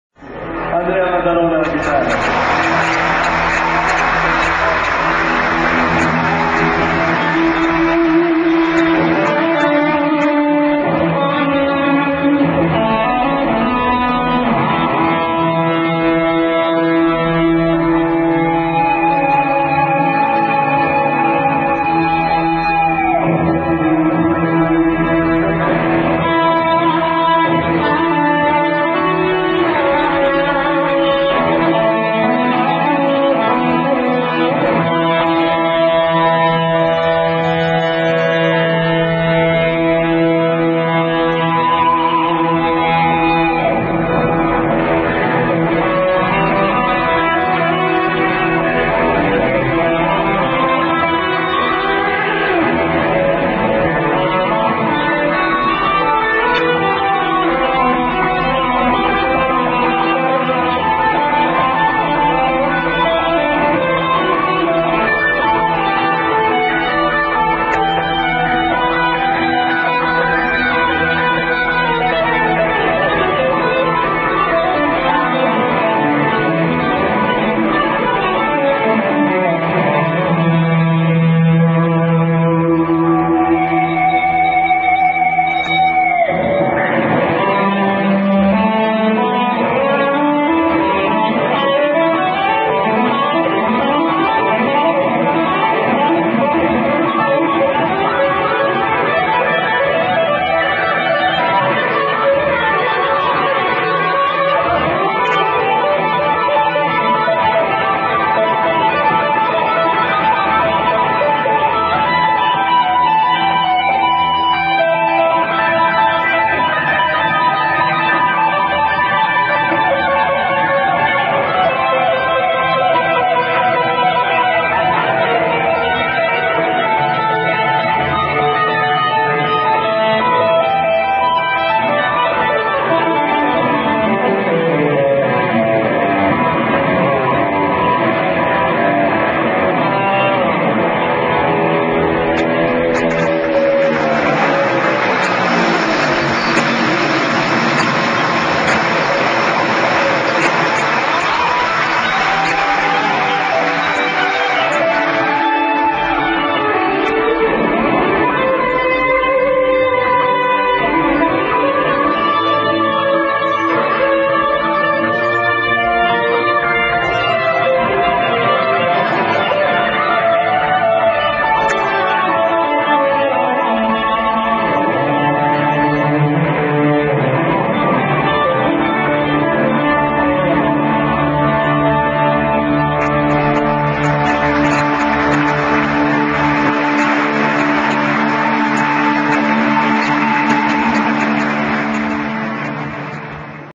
Andrea Maddalone's solo with New Trolls
Just watch the style and passion of this master of 6 strings, performing a solo during a New Trolls show. Andrea Maddalone, a "star of Italy" that shines bright